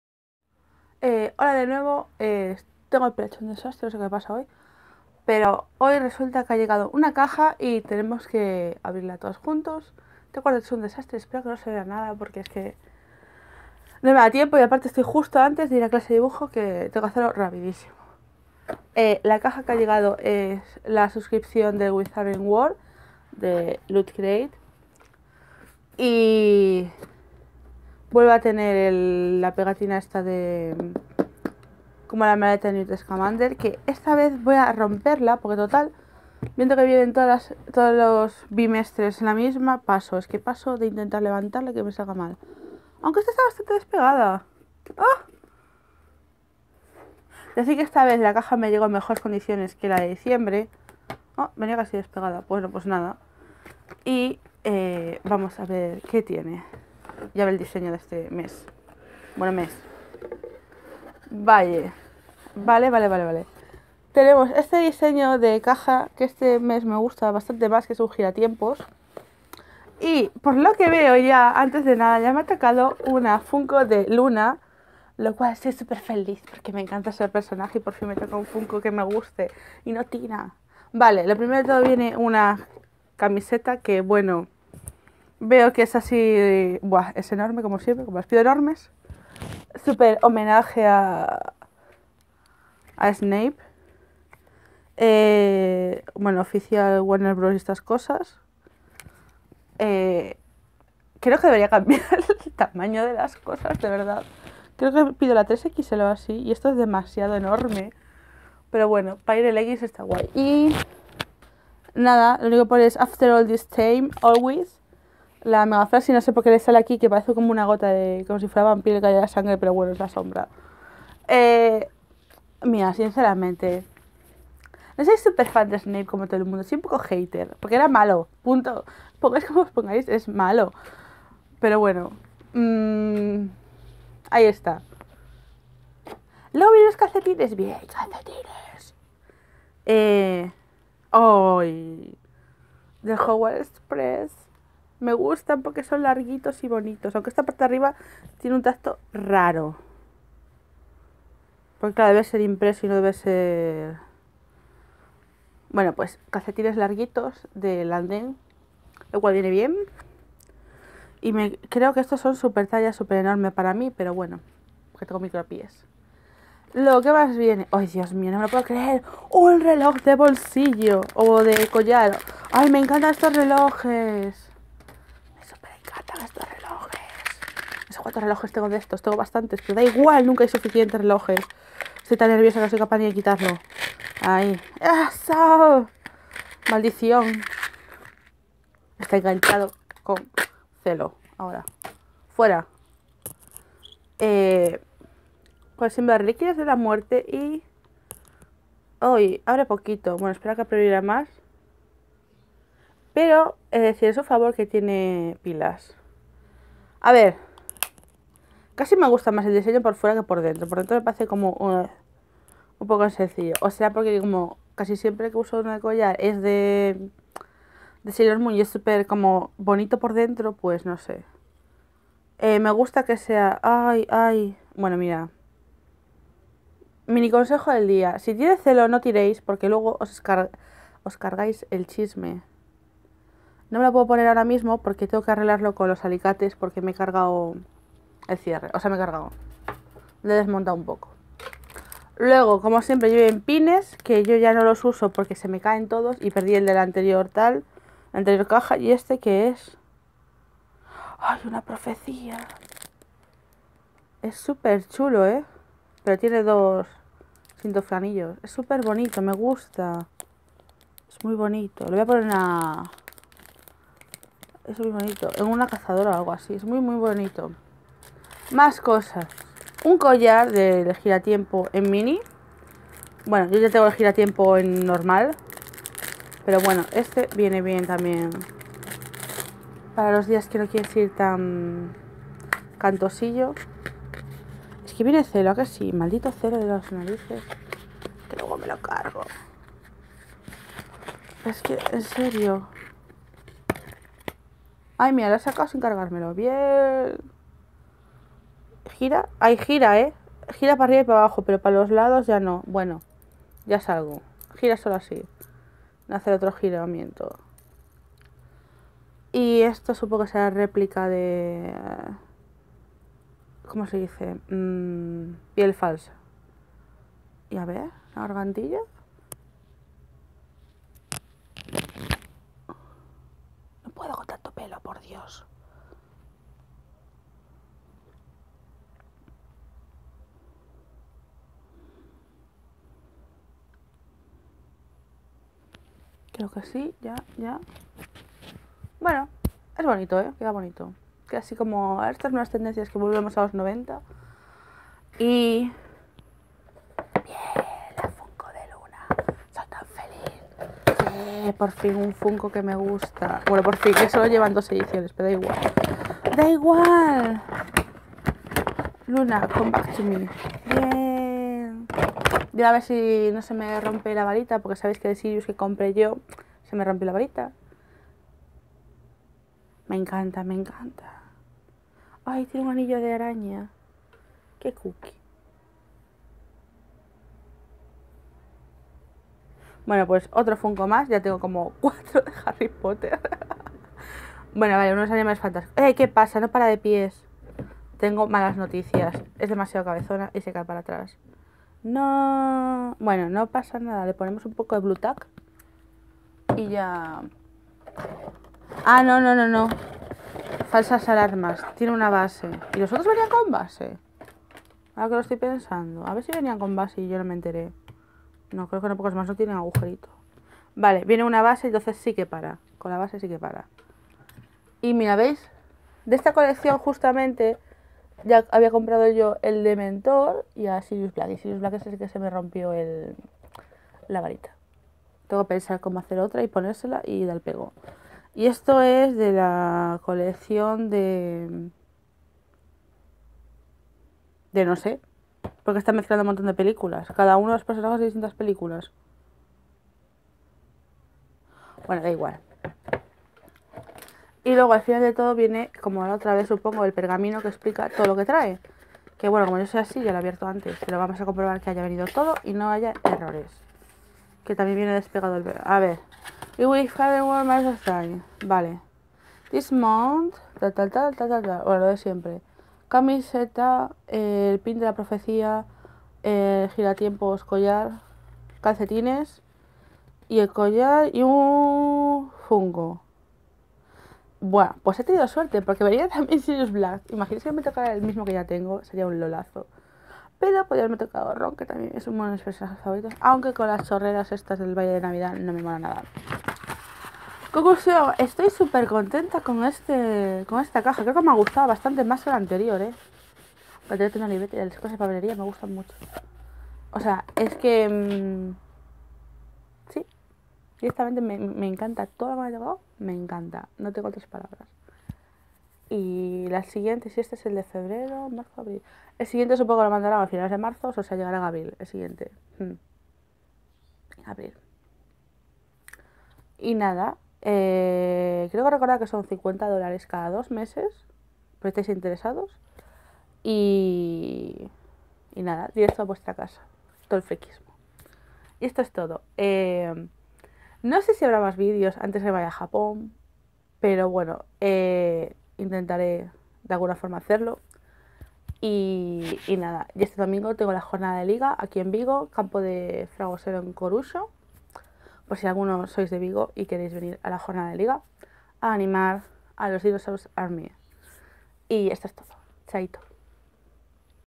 Lootcrate: Wizarding World febrero 2017
Nueva caja sorpresa del mundo de Jotaká. ¿Qué traerá esta vez? Descubrámoslo juntos.

Este vídeo no tiene ni edición porque se me ha estropeado el Premiere y estoy reinstalándolo ahora mismo.